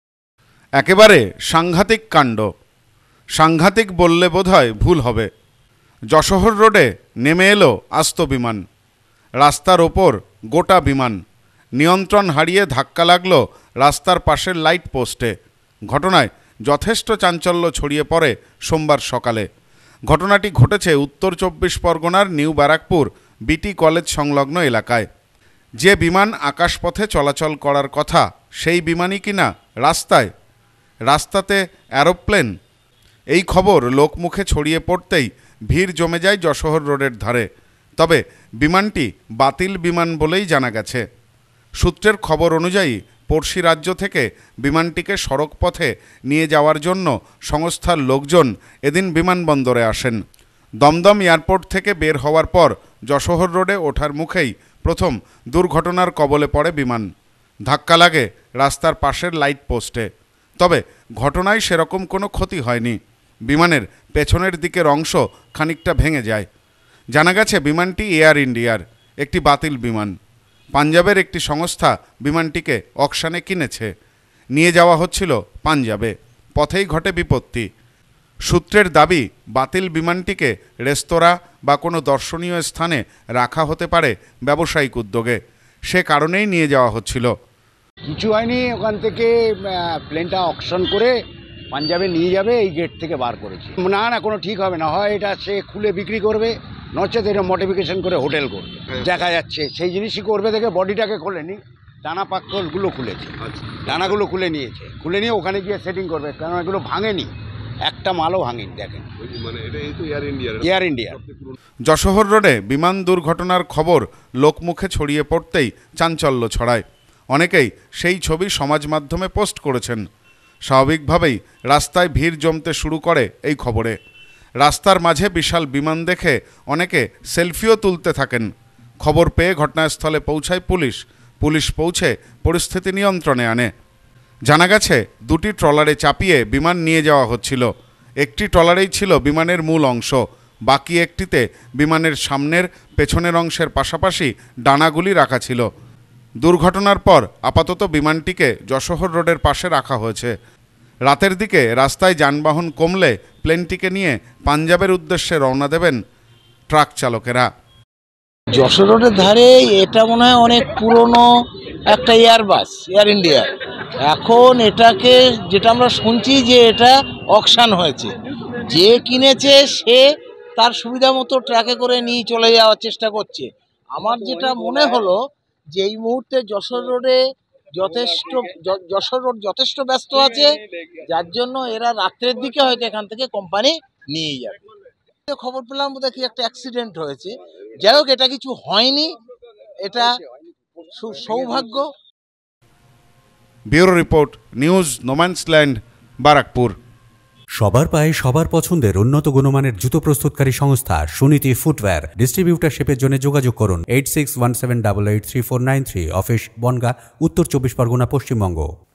যশোহর রোডে দুর্ঘটনার কবলে বিমান!
যশোহর রোডে দুর্ঘটনার কবলে বিমান! প্লেন দেখতে রাস্তার ধারে ভিড় সাধারণের৷ সেলফি তোলার হিড়িক৷ আপাতত রাস্তার পাশে রাখা হয়েছে বিমানটিকে৷